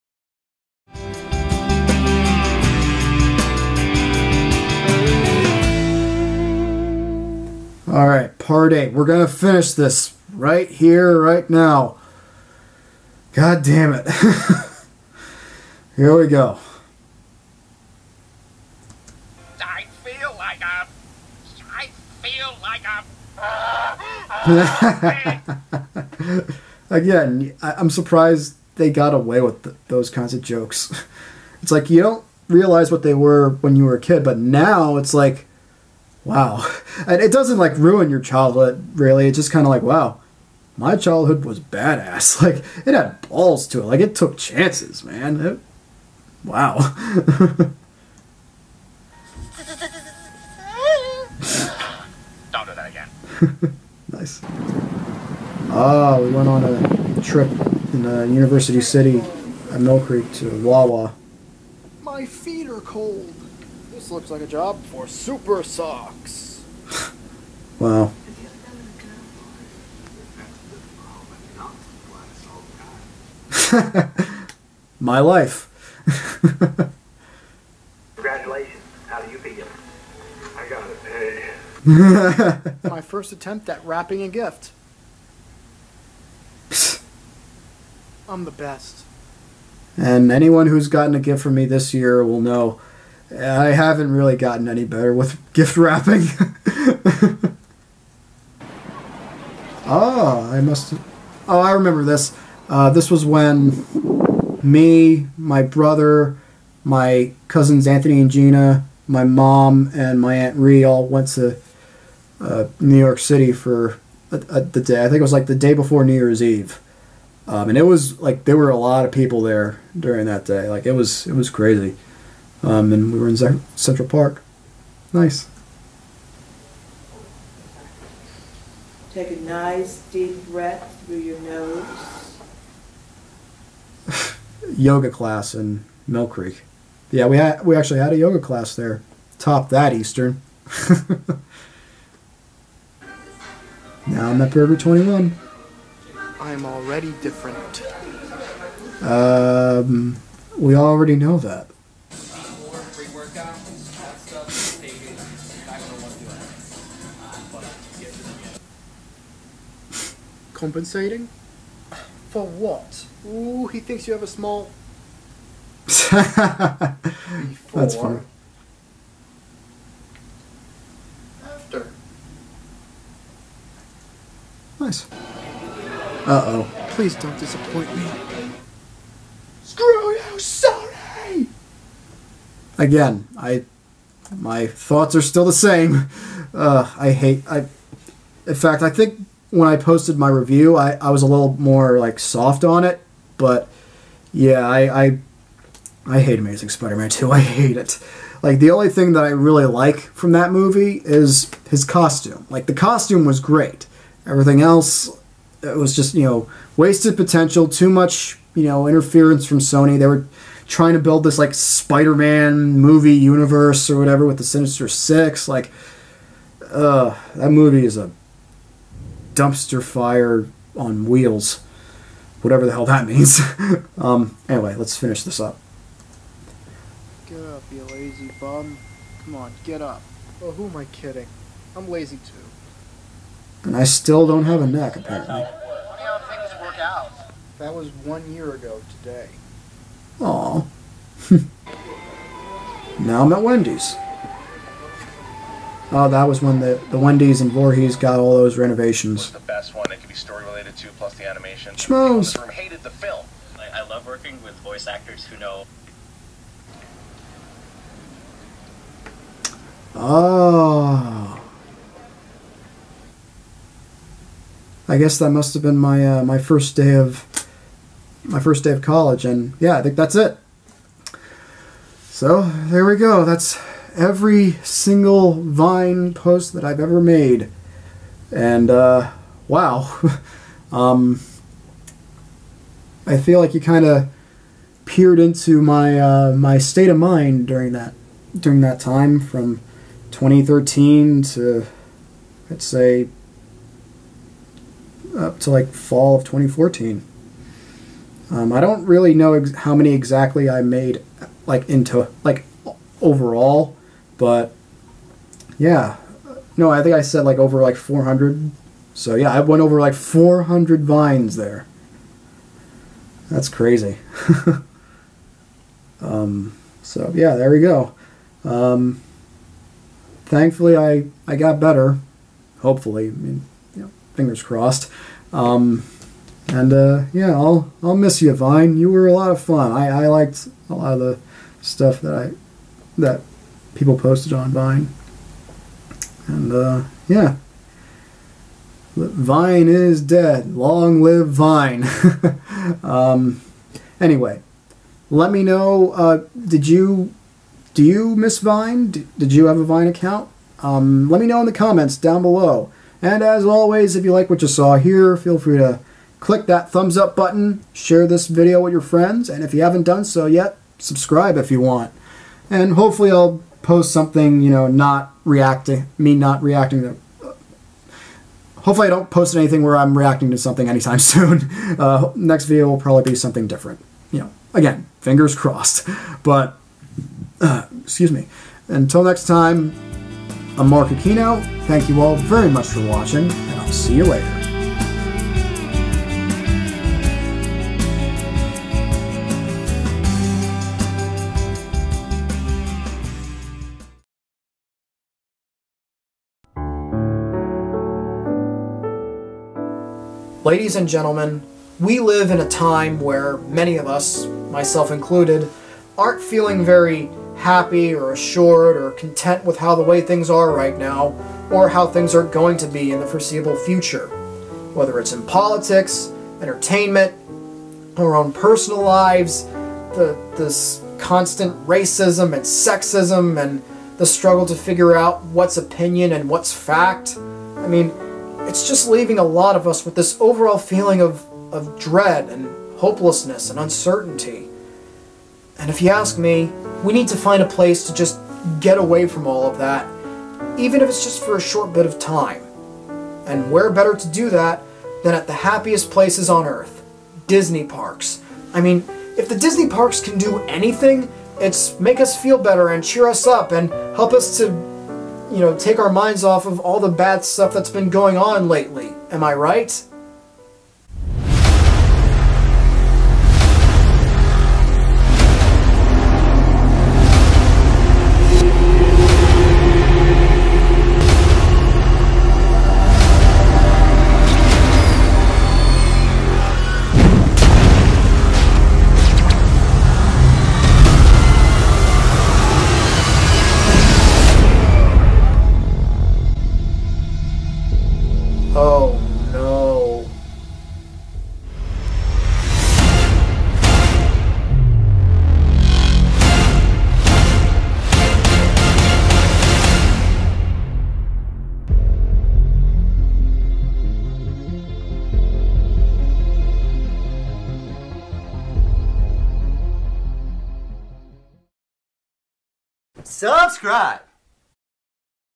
Marc Reacts To His Old VINE Posts Part 8
Marc finishes reacting to his old Vine posts, and it's still embarrassing.